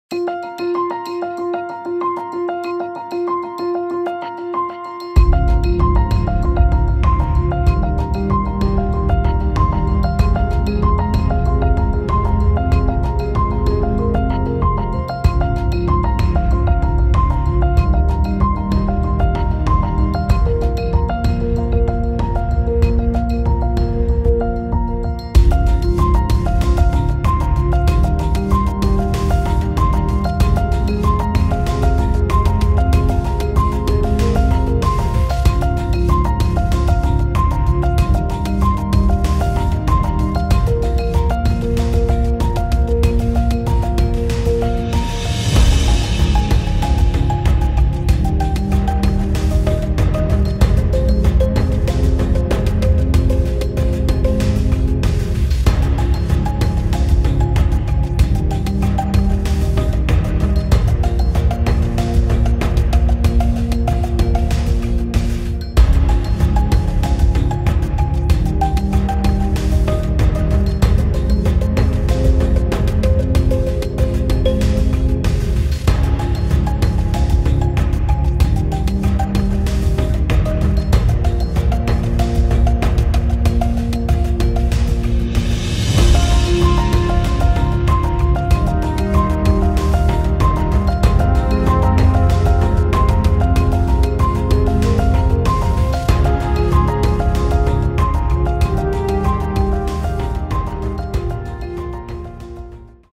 New York Post insists on using Saudi Defense Minister's photo on unrelated statements.
Comments accusing the website of ill will towards Saudi Arabia. New York Post insists on using Saudi Defense Minister's photo on unrelated statements.